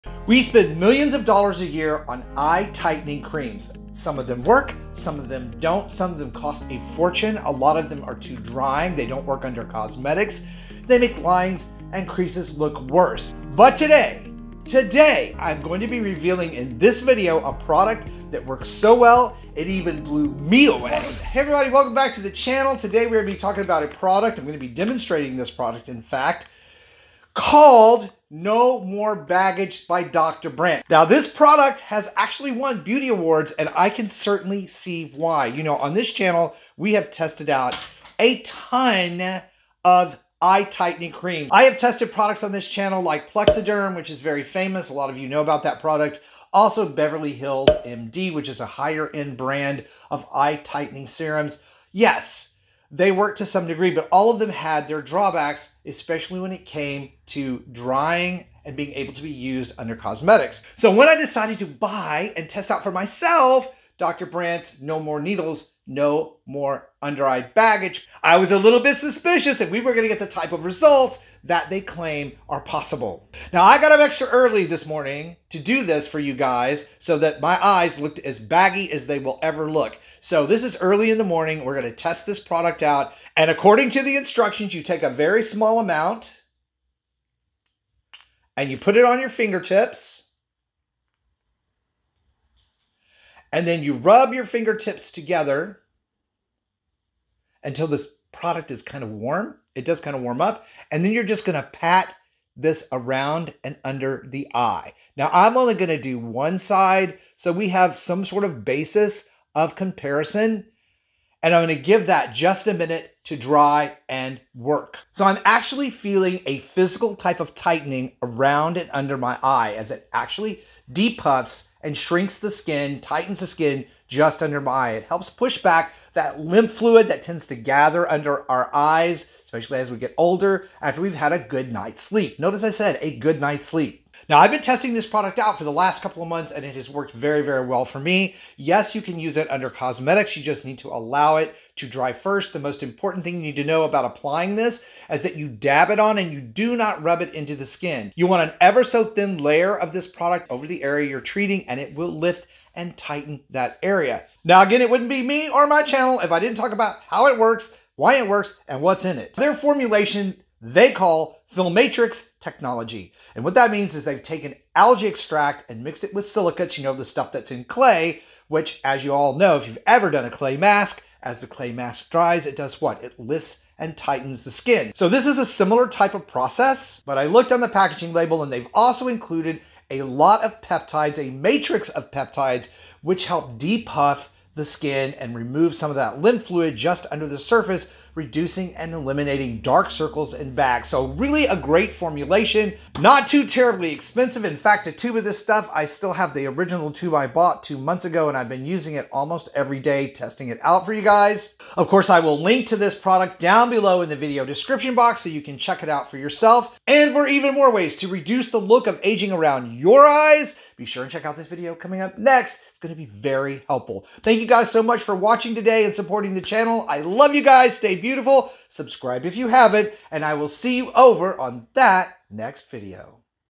CRAZY RESULTS Best Eye Lifting And Firming Cream Ever? | Chris Gibson
This eye lifting and tightening cream gel is one of the best I have ever tried!

✔️Products I mention in this video:
Dr. Brandt No More Baggage Eye Gel: $28 0n Amazon!